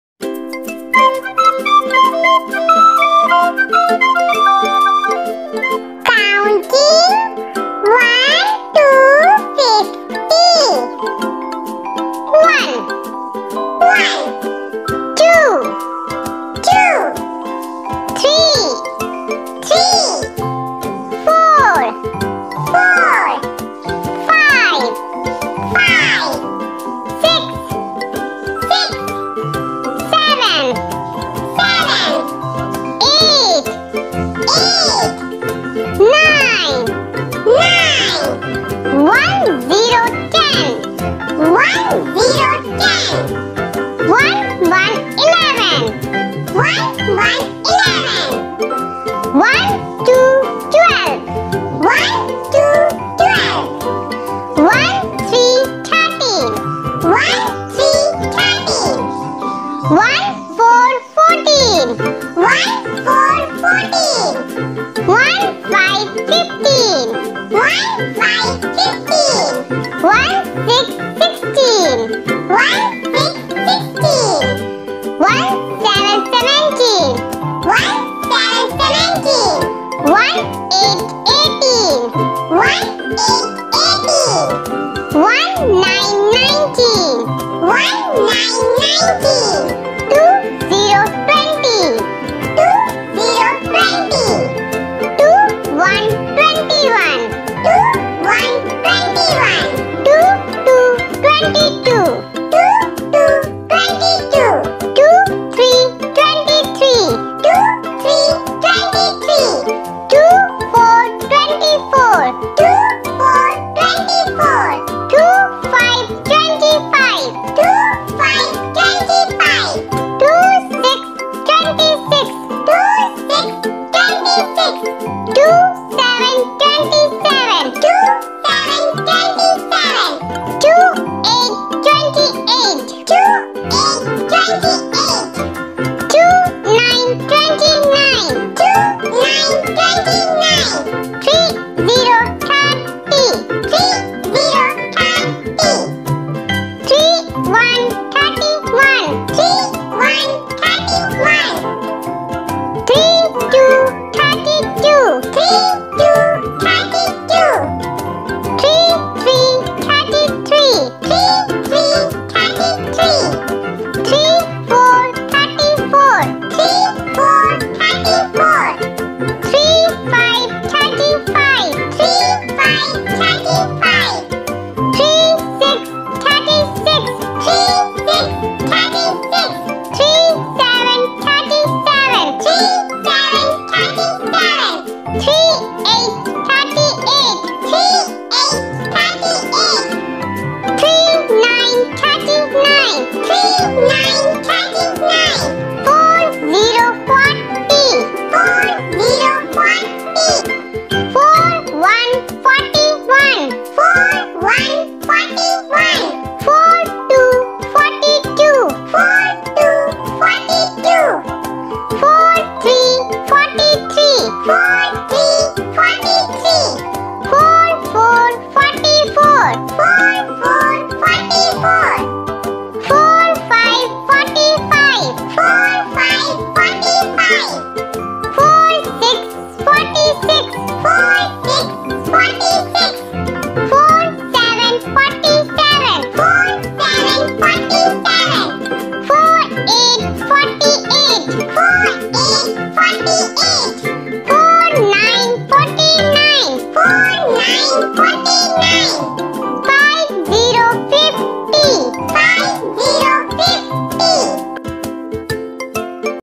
Learn to Write Numbers From 1 to 50/How to write Numbers From 1 to 50/Preschool Learning Videos
I tiny Kids Here Are Another Video OF Learning How To Write Numbers From 1 to 50
We made Our Videos In a Animated And Easy Way So kids Can Learn Easily To Understand And LEarn Quickly
If You Really Like Our Videos Please Share Some Love And Support To My Channel